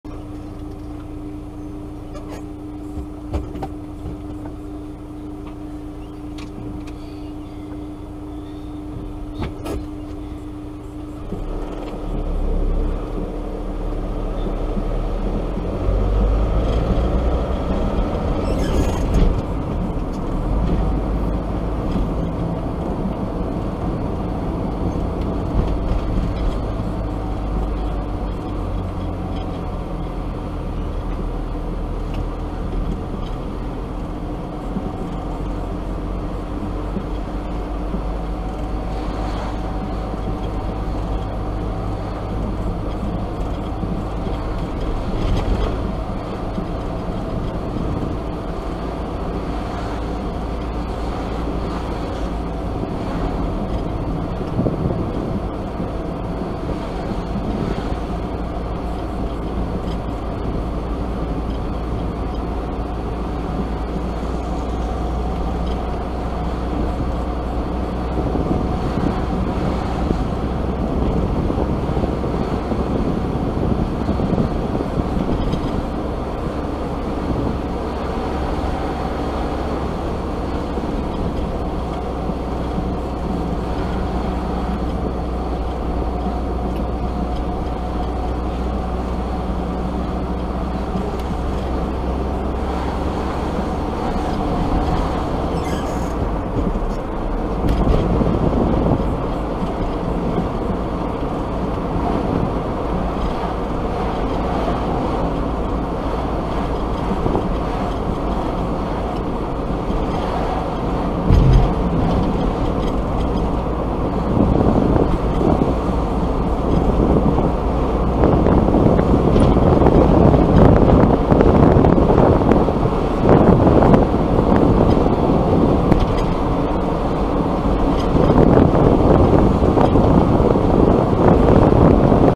Snapshot of hill stabilisation work A83 Rest and Be Thankful pass 26.7.21 Arrochar Argyll Scotland
Debris regularly falls over a thousand feet onto the road in times of bad weather.